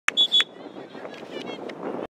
Hellgate boys varsity soccer lose to Glacier 0-1 at an away game on Thursday, September 17th.